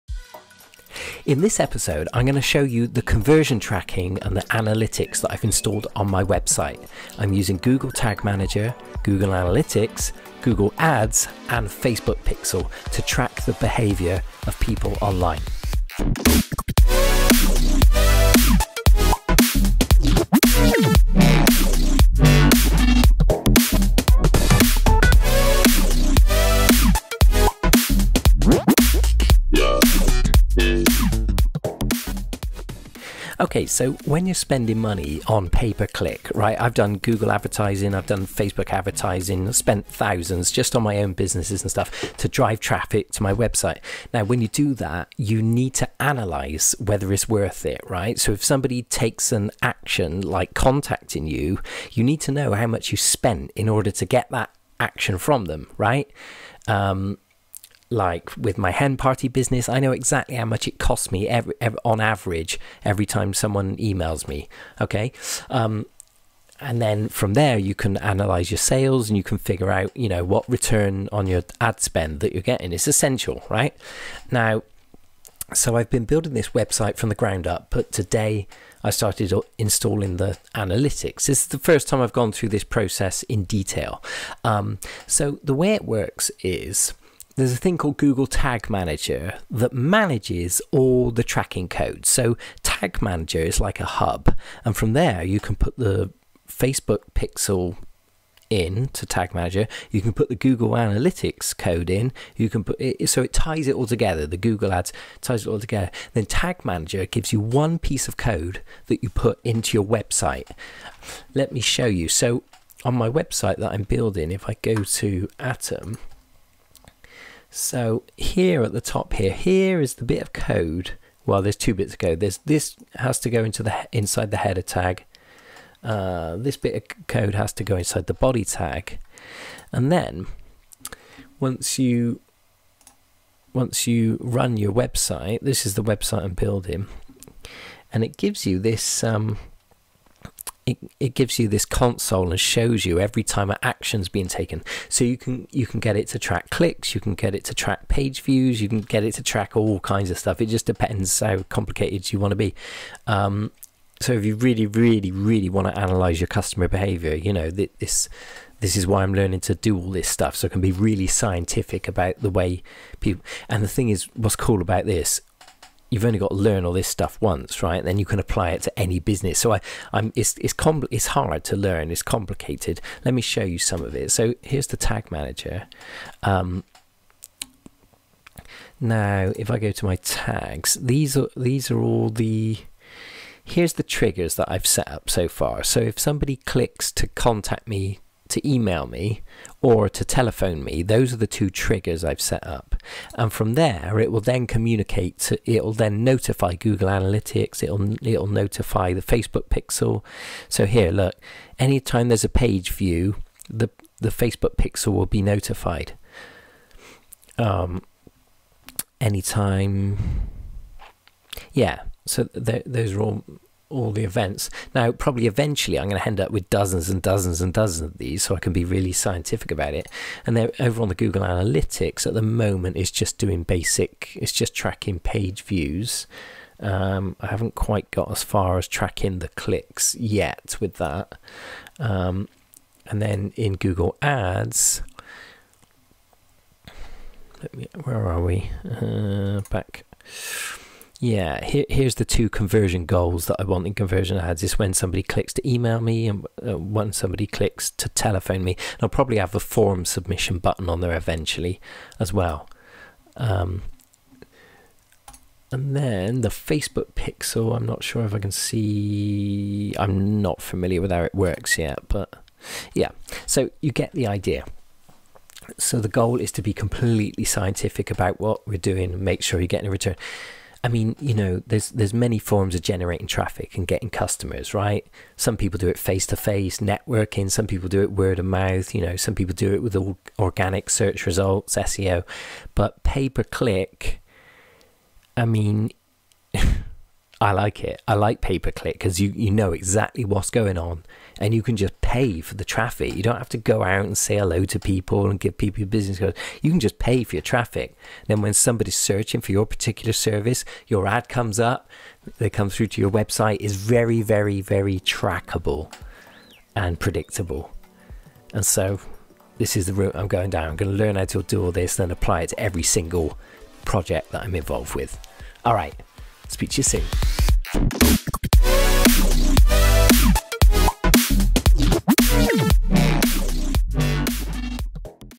#019 - Tag Manager, Conversion Tracking, Google Analytics, Ads, Facebook Pixel
In this epsiode I show you how i'm getting super scientific with tracking constomer behaviour and cost per conversion with Google Ads, Google Analytics, Googl Tag Manager, and Facbeook Pixel.